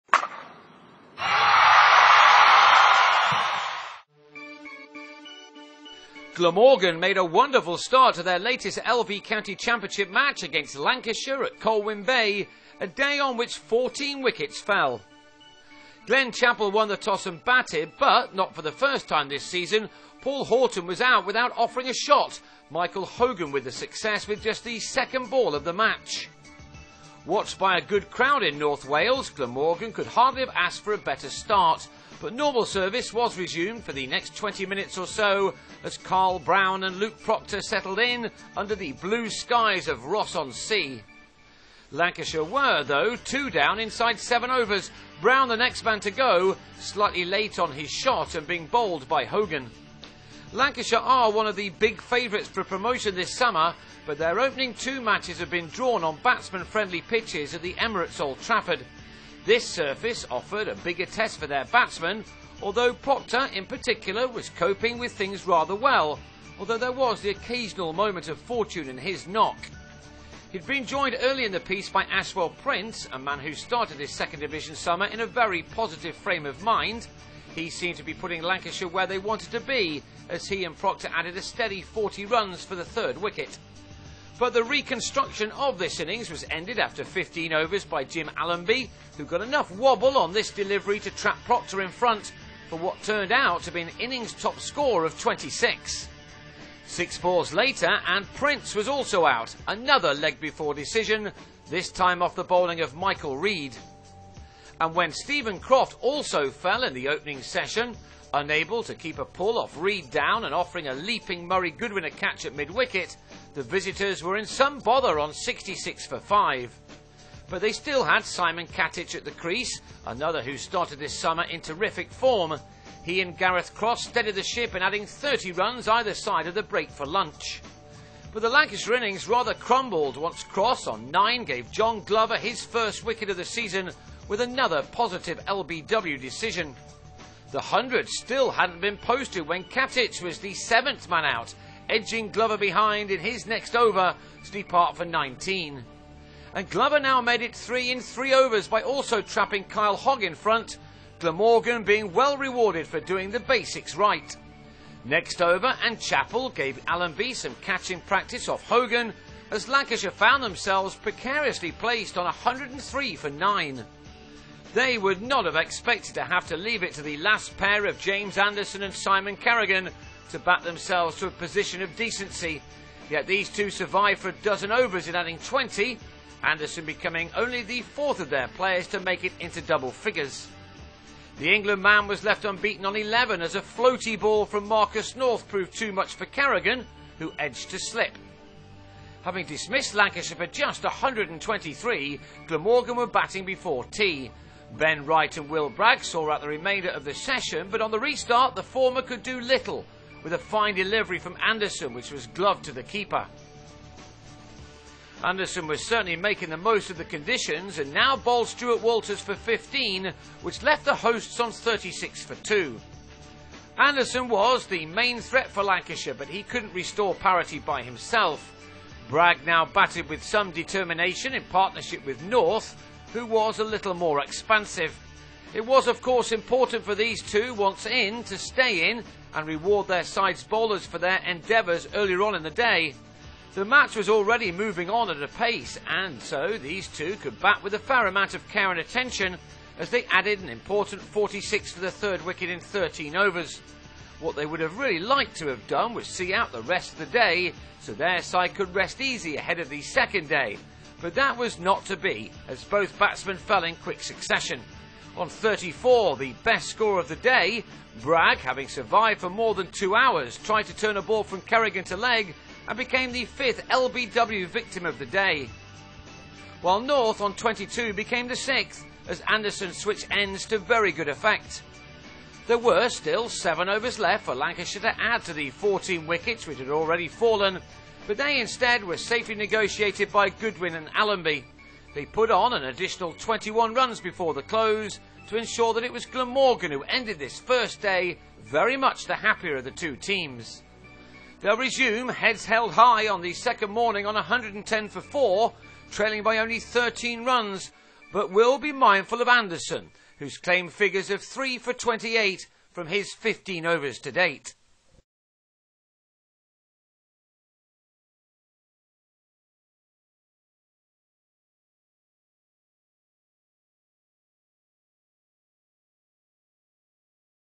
Glamorgan v Lancashire at Colwyn Bay Day 1 Highlights
Glamorgan face Lancashire including James Anderson in their ranks for the LV= County Championship clash at Colwyn Bay